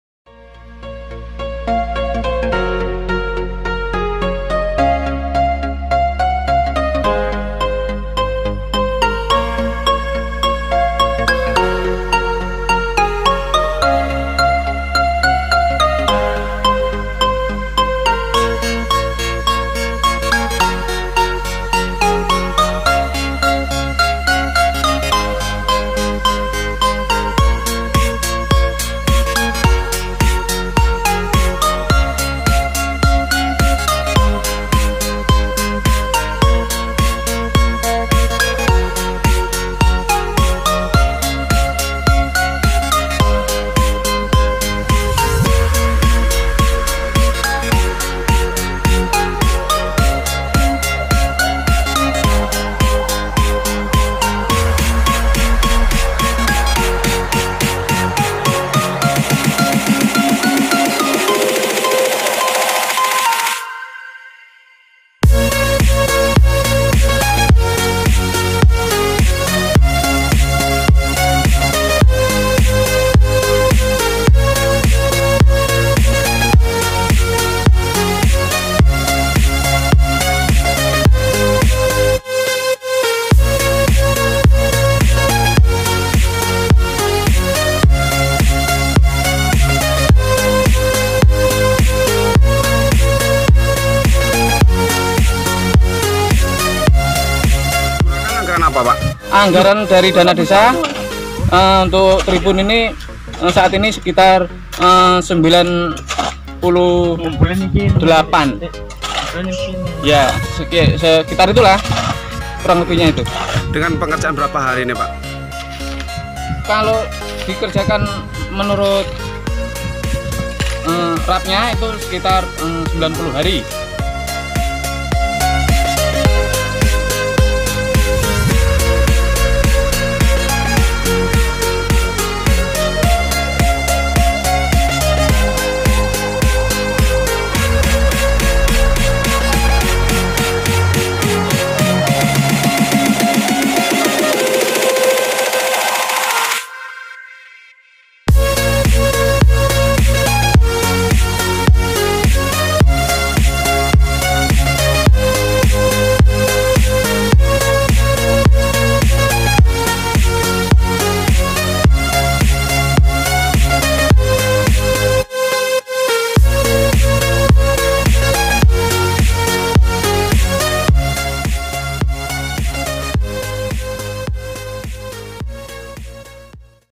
MENUJU KEMAJUAN DESA
Ini adalah dokumentasi proses pembangunan podium dari awal sampai finish.
Podium yang dibuat pada akhir 2019 ini,semoga saja bisa membuat warga dan pemuda bangga.akan keindahan daerahnya sendiri.